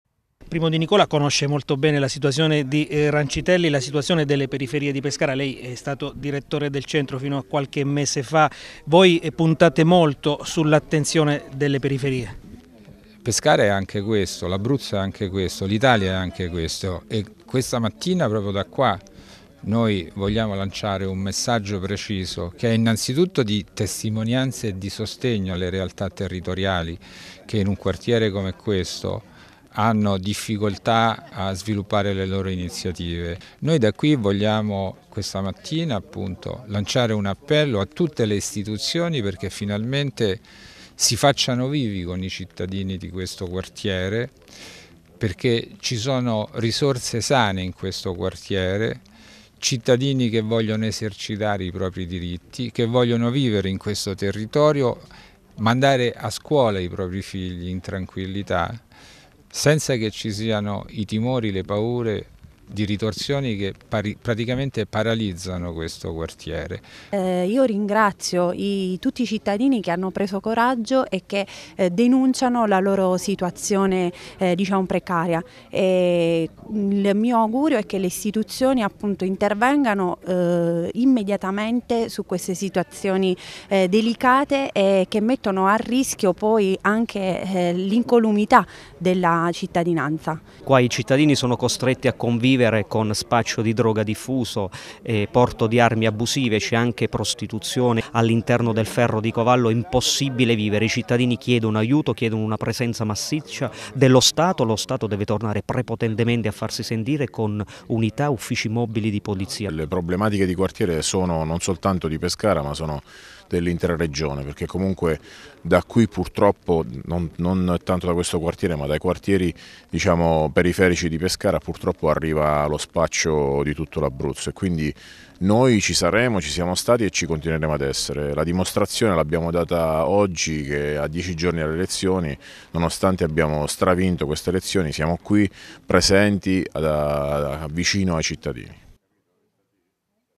Ordine pubblico a Rancitelli. La ricetta del M5S
Sempre più difficile la situazione ordine pubblico nel quartiere Rancitelli di Pescara. La denuncia arriva dal M5S che chiede interventi agli organi preposti e propone